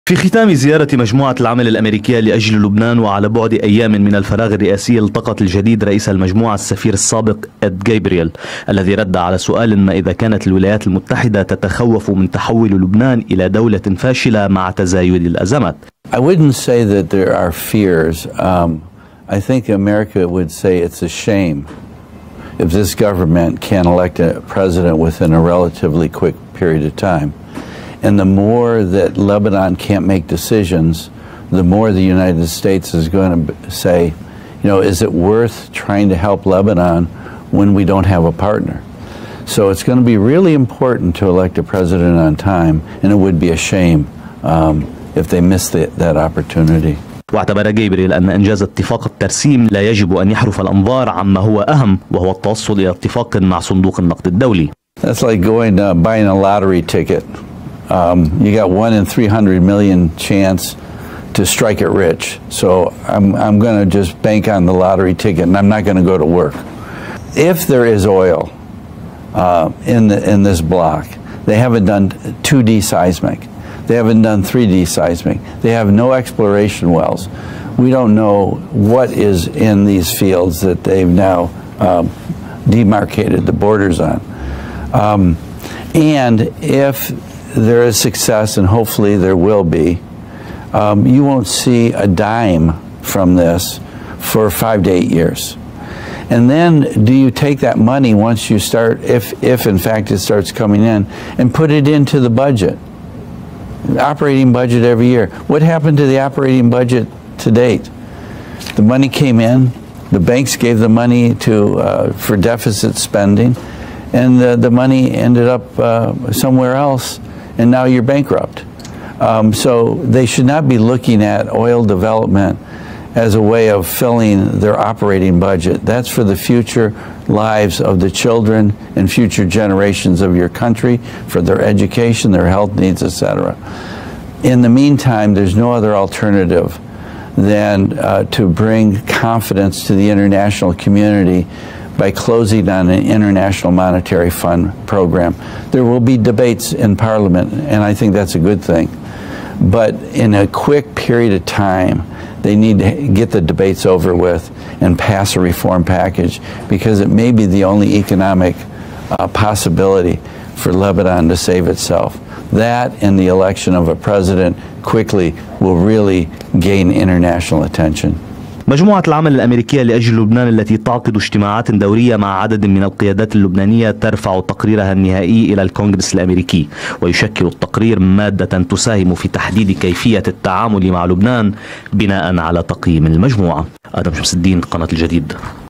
رئيس مجموعة العمل الأميركية لأجل لبنان : من المخزي عدم انتخاب رئيس للجمهورية
في ختام زيارة مجموعة العمل الأميركية لأجل لبنان وعلى بعد ايام من الفراغ الرئاسي التقت الجديد رئيس المجموعة السفير السابق إد غبريال الذي رد على سؤال ما اذا كانت الولايات المتحدة تتخوف من تحول لبنان الى دولة فاشلة مع تزايد الازمات